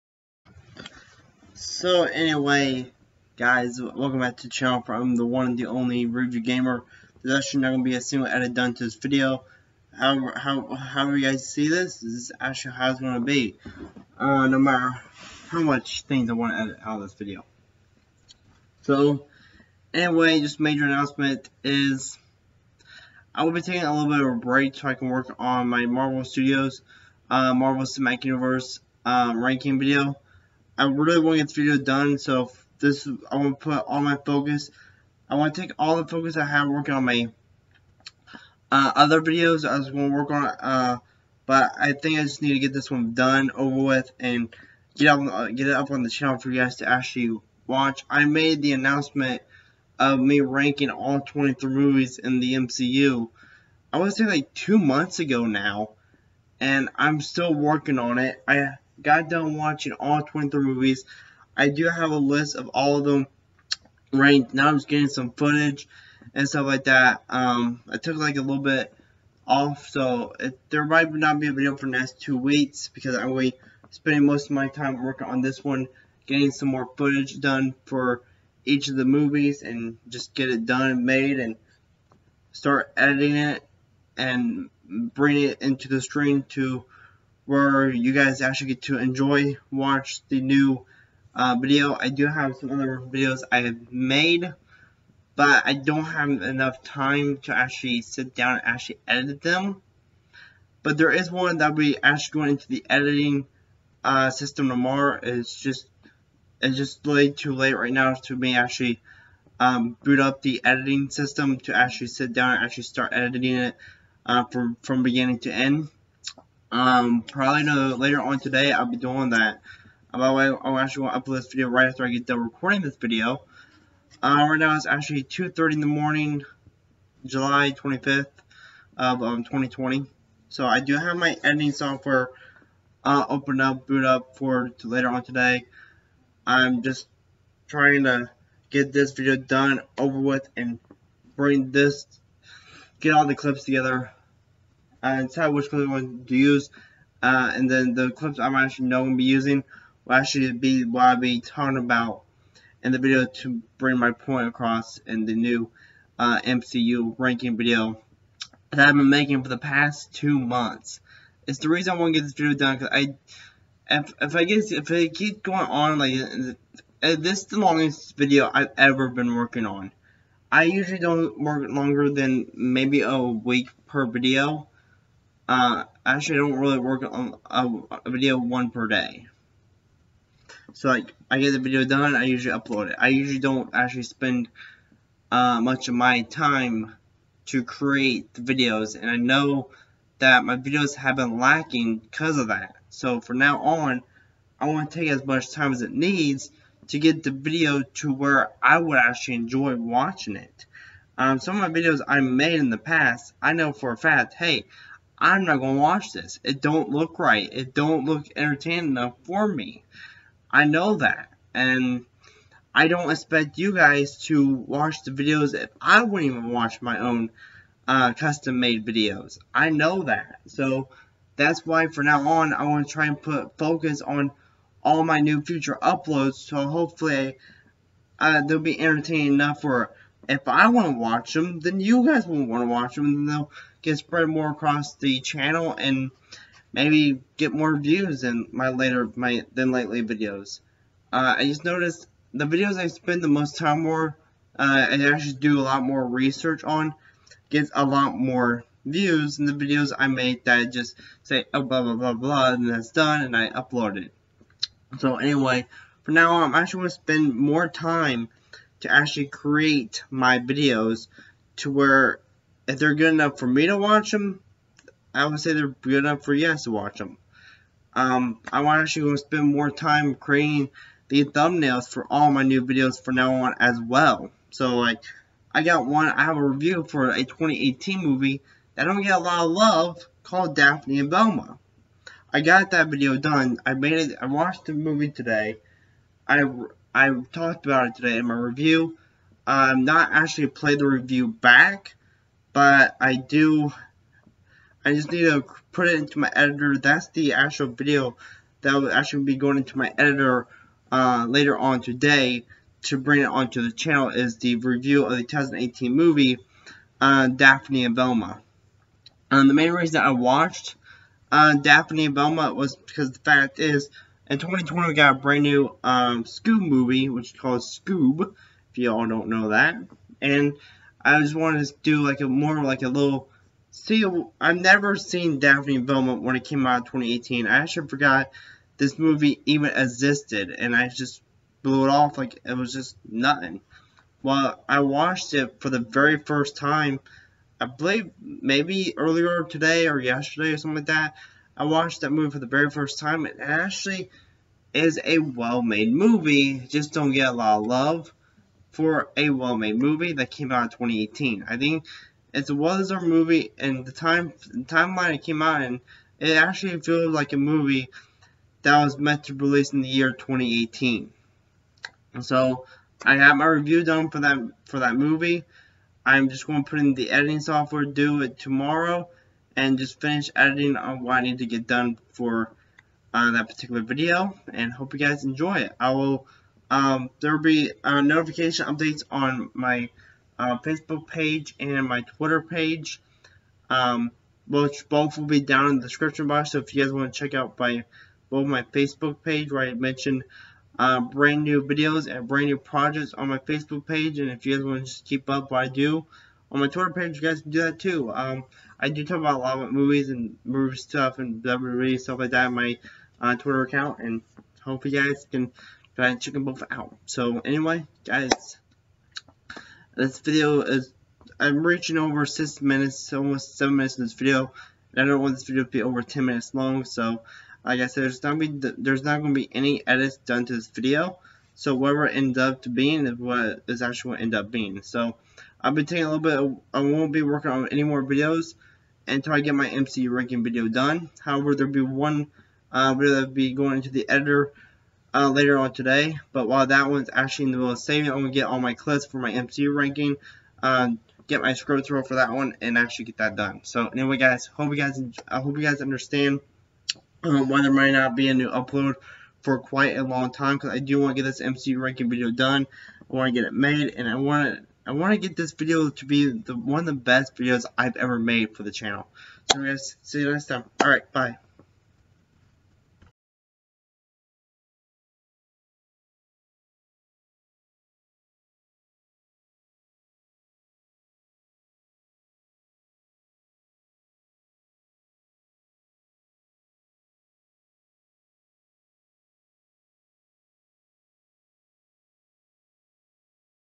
Video Announcement (ALL MY FOCUS IS ON THE MCU RANKING)
THIS IS THE VIDEO ANNOUNCEMENT  ON THAT ALL MY FOCUS HAS GONE TO IN THE MAKING OF THE MCU RANKING VIDEO AND FOR NOW ON I WILL KNOW WHAT VIDEO I NEED TO GET DONE FIRST

STUDIO NO STUDIO
MAIN ACTOR JONATHAN YOUNG
LINK TO GET THIS TITLE FROM AMAZON

STARTING WITH THE NEXT VIDEO I WILL HAND MAKE ALL MY THUMBNAIL TOO MAKE THEM STAND OUT MORE